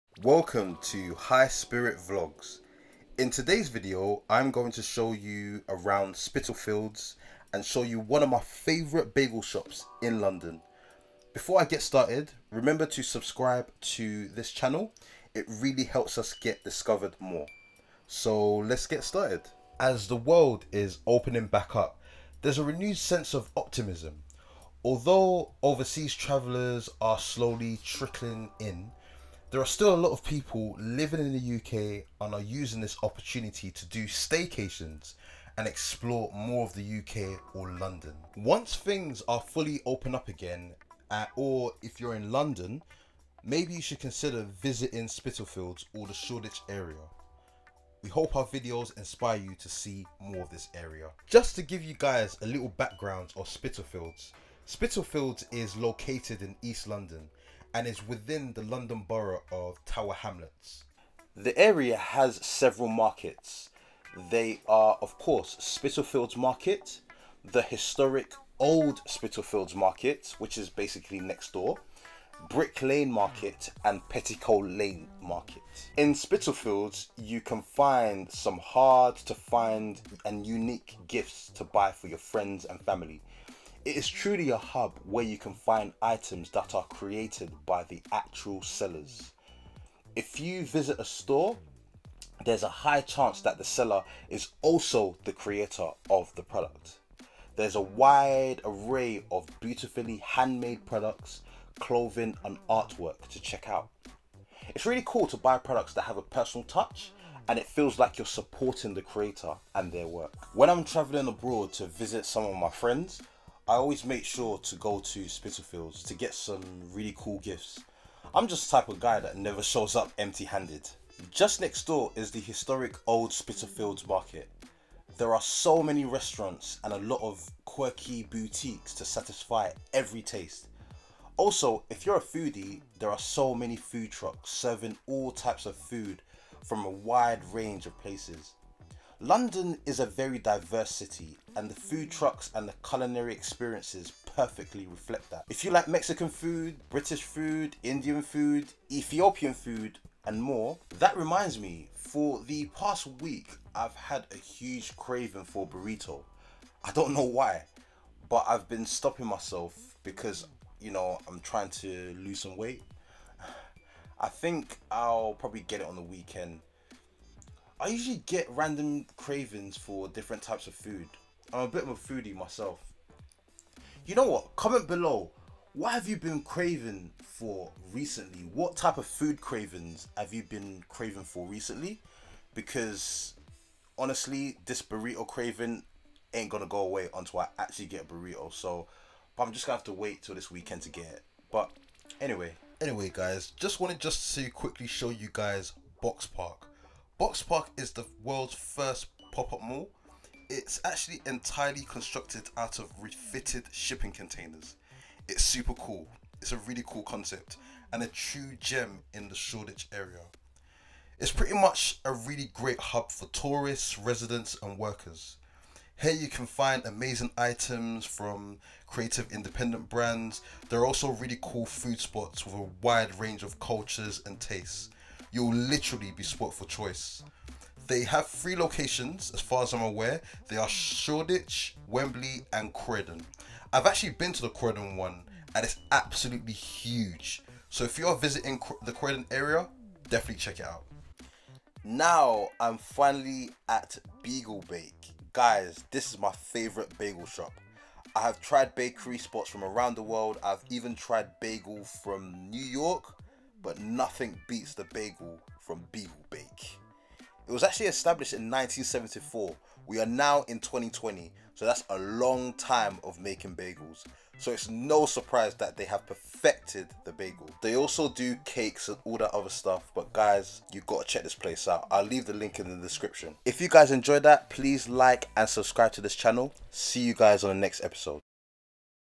Spitalfields Market and WalkThrough and Beigel Bake Visit - Reopening London
In this video, I do a Spitalfields Market walkthrough. I also visit Biegel Bake and briefly show you Boxpark Shoreditch. I had a craving for a plain Beigal Bake Bagel so I show you all the journey to get there while showing some sites of East London.

Here is the link to register for FREE entry to Boxpark, Shoreditch. To avoid disappointment, make sure you register before you go.